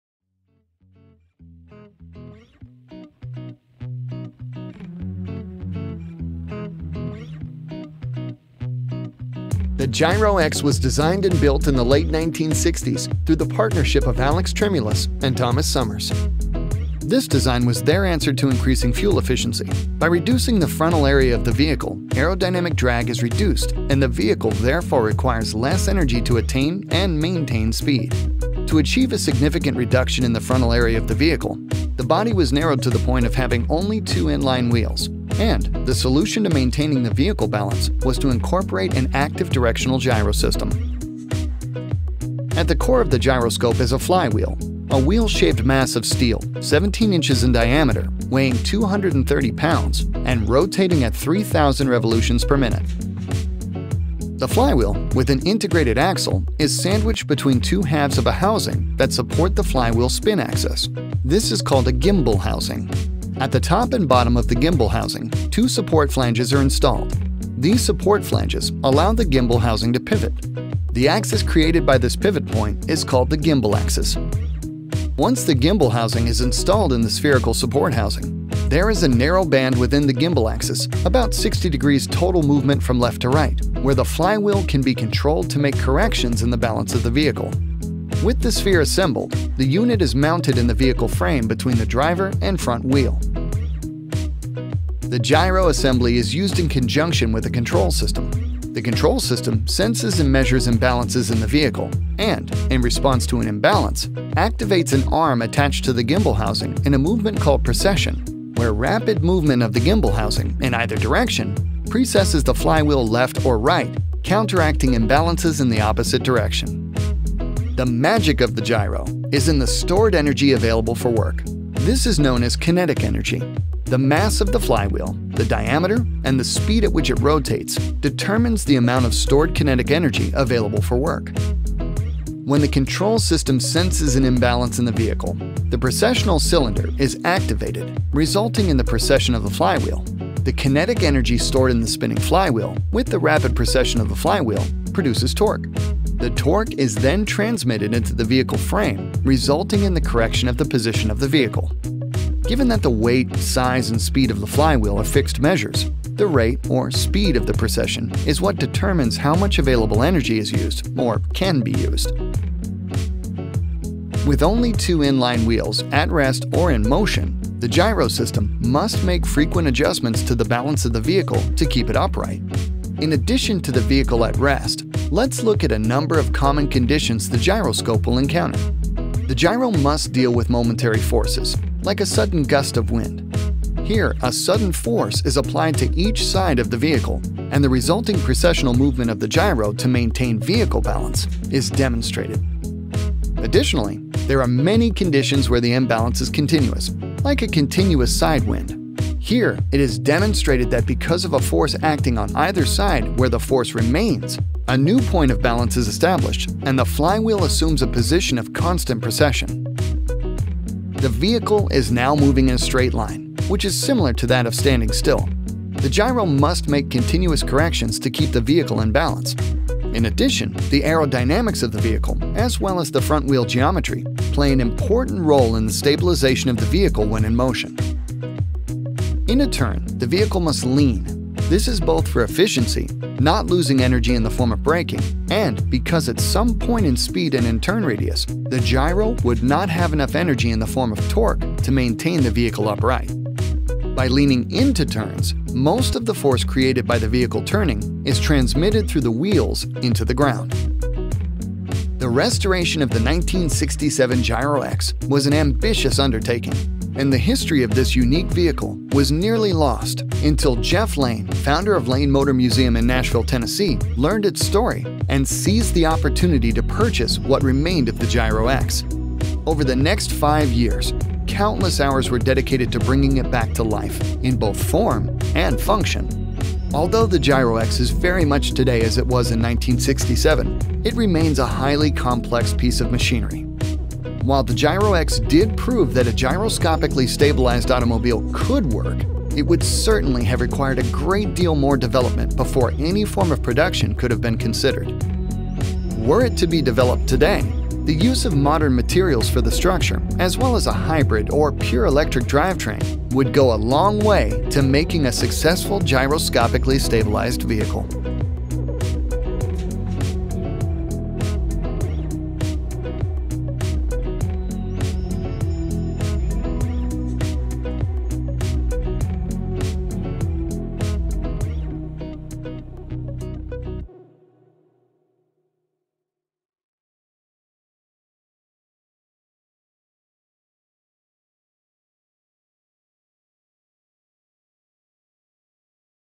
Lane Motor Museum explains their 1967 Gyro-X
The brainchild of Alex Tremulis, famous stylist and Automobile Hall of Fame inductee, and Thomas Summers, a gyroscope expert, the Gyro-X is a two-wheeled, gyroscopically-stabilized prototype vehicle constructed in 1967. Proposed as a possible solution for future transportation, the two-wheeled vehicle provided many thought-provoking ideas for revolutionizing transportation.